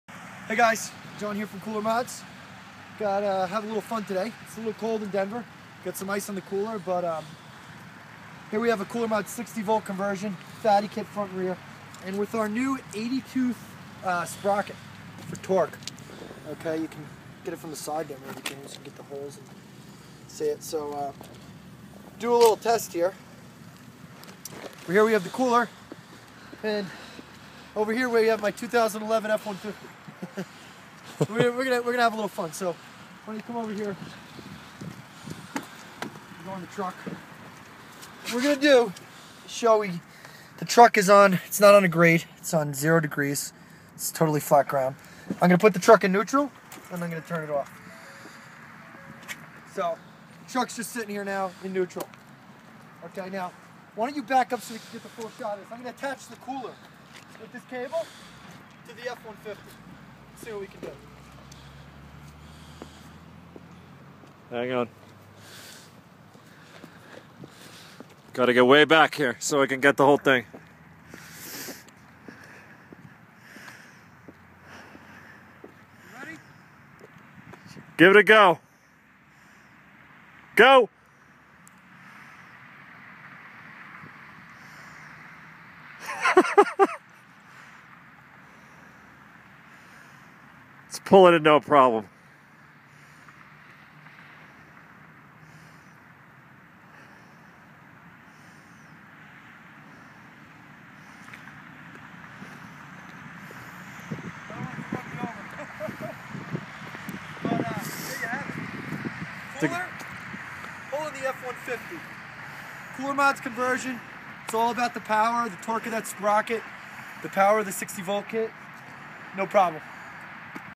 CoolerMods Cooler pulls F150 See coolermods.com for more info
for more info!

Coolermods Cooler with 60V Conversion, Fatty Kit front and Rear, and 80 tooth Coolermods sprocket shows its torque by pulling a 2011 Ford F150.